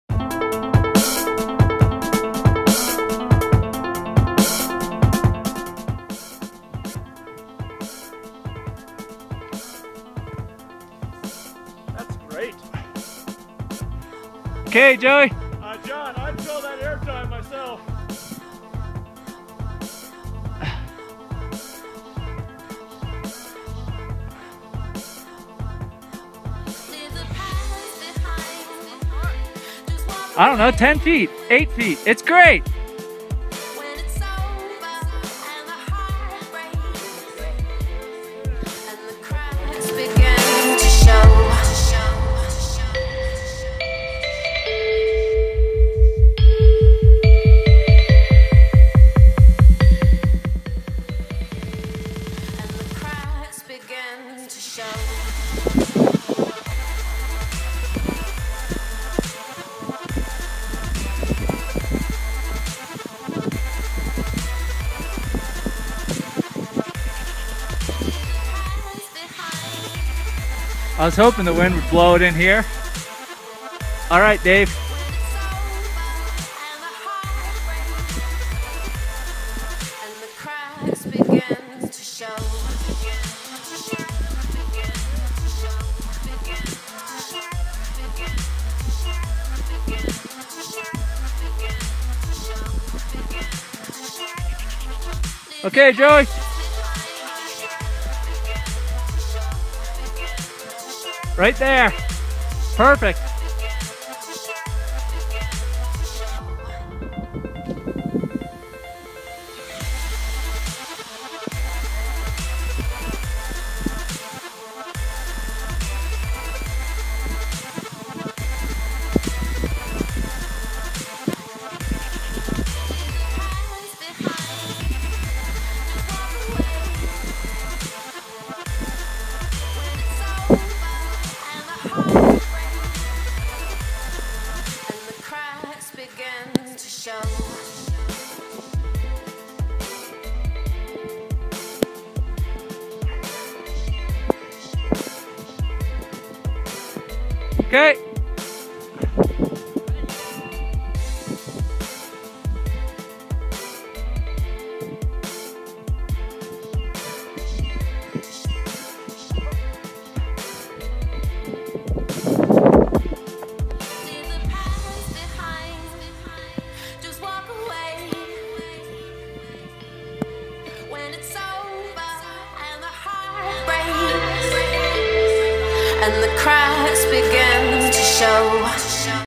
Skiing RMNP Powder 4/17/13
We had an amazing tour in RMNP today. Deepest powder of the year.